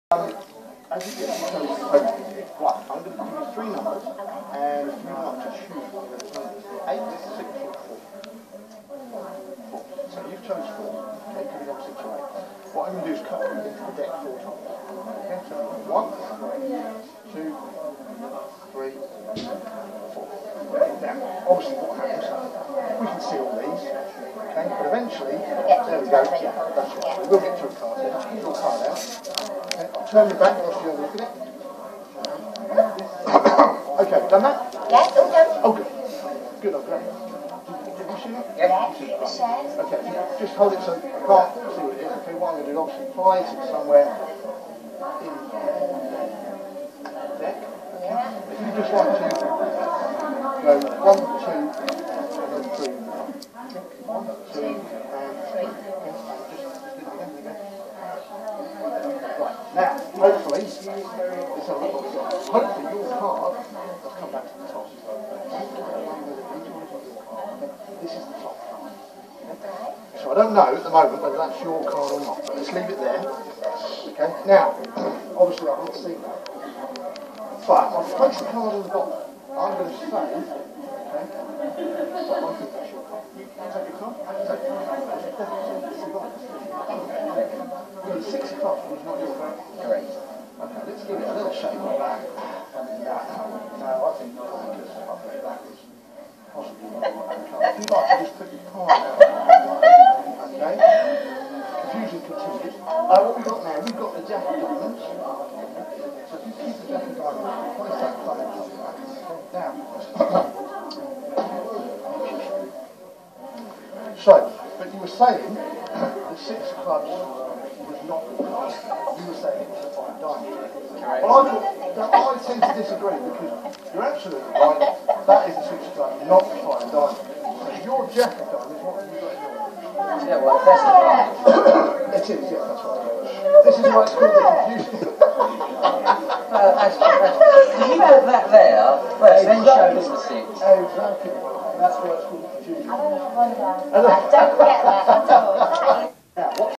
Magic Entertainer
Paul Newman Card Magician performing at a Wedding fare at The Holiday inn Farnborough